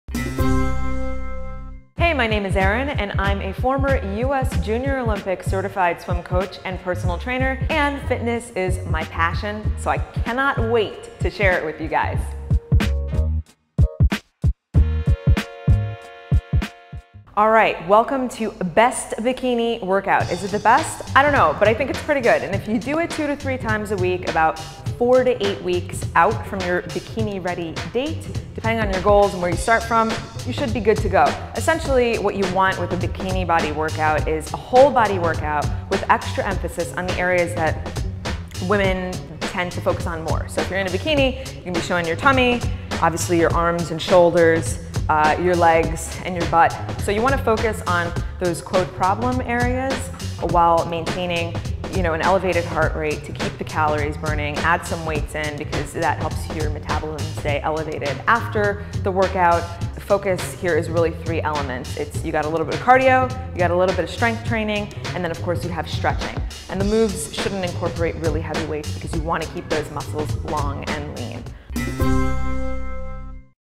How to Do the Best Bikini Workout | Female Bodybuilding
like these Workout Lessons !!!

Gaiam Total Body Balance Ball Kit
Jillian Michaels: No More Trouble Zones
Fitbit Flex Sleep + Activity Tracker

Hey, my name is Erin and I'm a former US Junior Olympic certified swim
coach and personal trainer and fitness is my passion so I cannot wait to
share it with you guys.

Welcome to best bikini workout.  Is it the best? I don't know, but I think
it's pretty good and if you do it two to three times a week about four to eight weeks out
from your bikini ready state depending on your goals and where you start
from, you should be good to go.  Essentially what you want with the bikini
body workout is a whole body workout with extra emphasis on areas that
women tend to focus on more.   So, if you're in a bikini and you're showing
your tummy, obviously your arms and shoulders, your legs and butt.  You
want to focus on those quote problem areas while maintaining an elevated
heart rate to keep the calories burning, add some weights because that
helps your metabolism stay elevated.  After the workout focus, there is
really three elements, there is a little bit cardio, a little bit strength
training and of course you have stretching and the moves shouldn't
incorporate really heavy weights because you want to keep those muscles
long and lean.